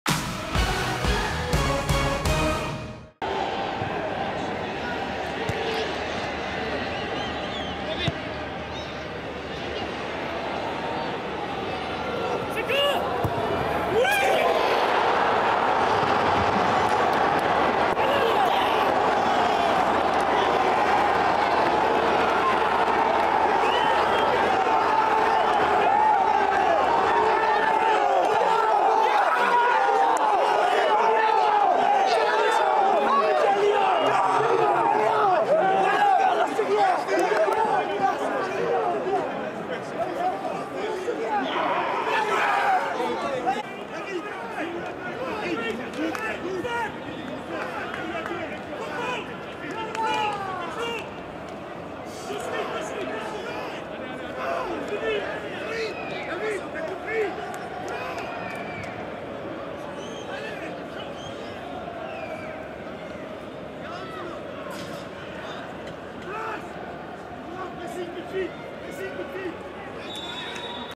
Goal Seko FOFANA (90' +5 - RCL) AS SAINT-ÉTIENNE - RC LENS (1-2) 21/22
"AS SAINT-ÉTIENNE - RC LENS (1-2) Seko FOFANA (90' +5)  goal. AS SAINT-ÉTIENNE vs RC LENS Goals in video

Ligue 1 Uber Eats - Season 2021/2022 - Week 21 STADE GEOFFROY-GUICHARD - Saturday 15 January 2022